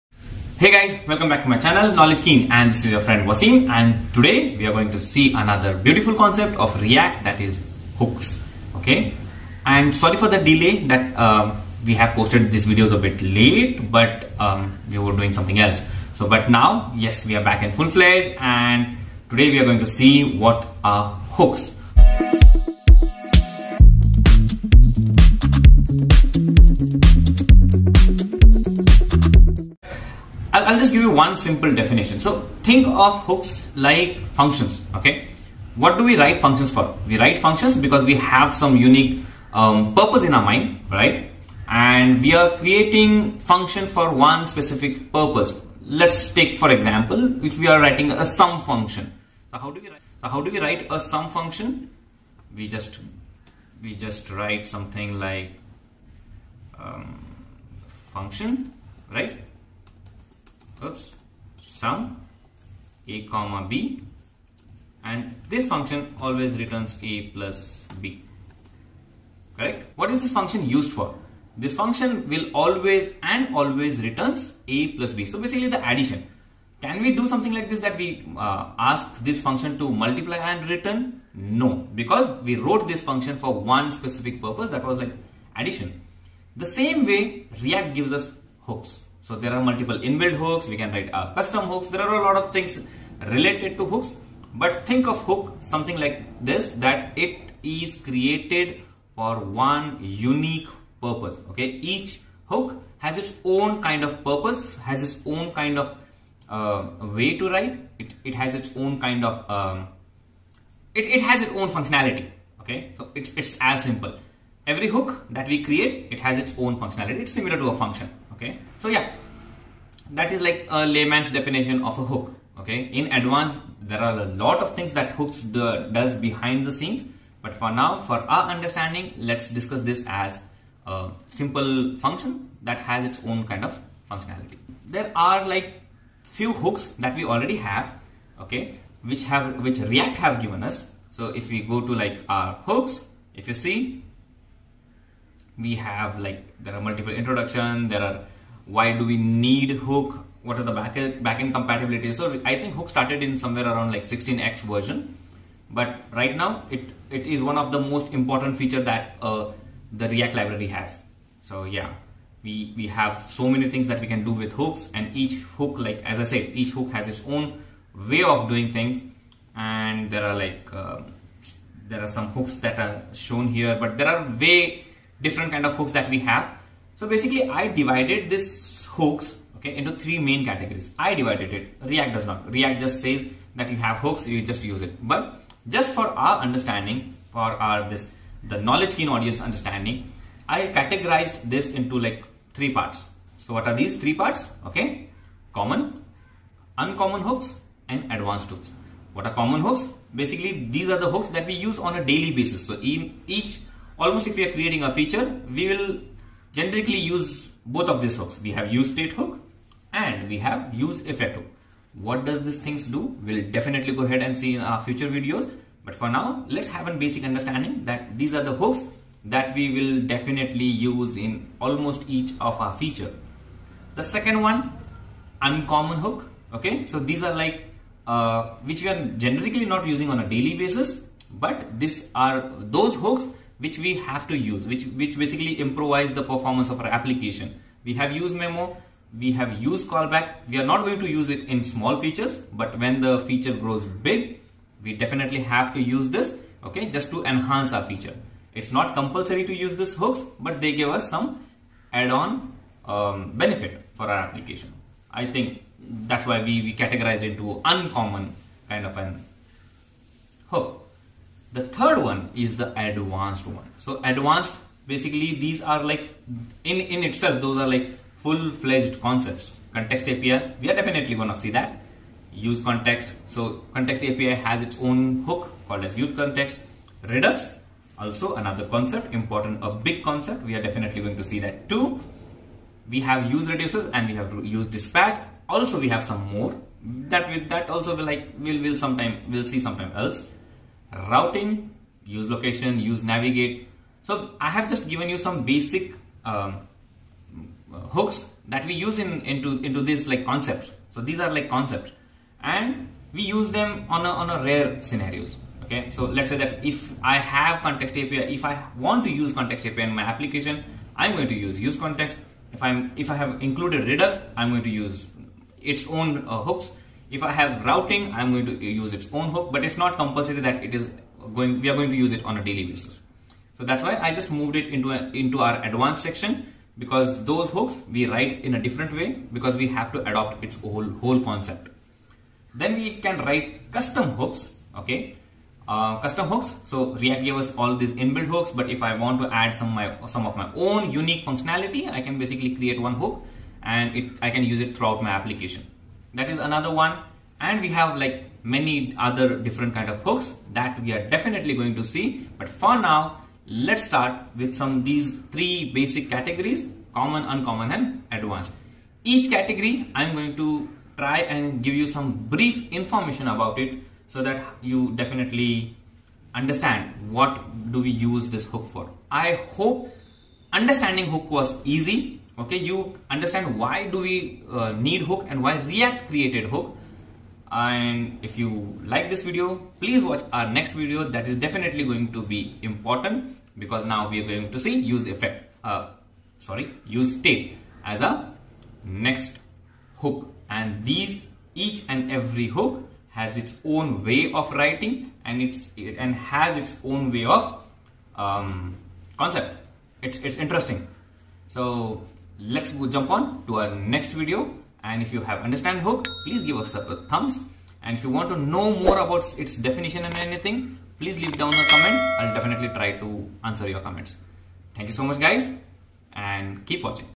Introduction to Hooks in React
"Hooks" as the name suggests - allows us to hook into React mindboggling functionalities. Let's understand what hooks are and how they work in React.
Hooks are similar to functions with a unique end goal. Hooks are a great way to add supplemental functionality to our React applications.
In the video, I have categorized the hooks into 3 parts so that we can understand the concept better.

Do share your views and comments on the video.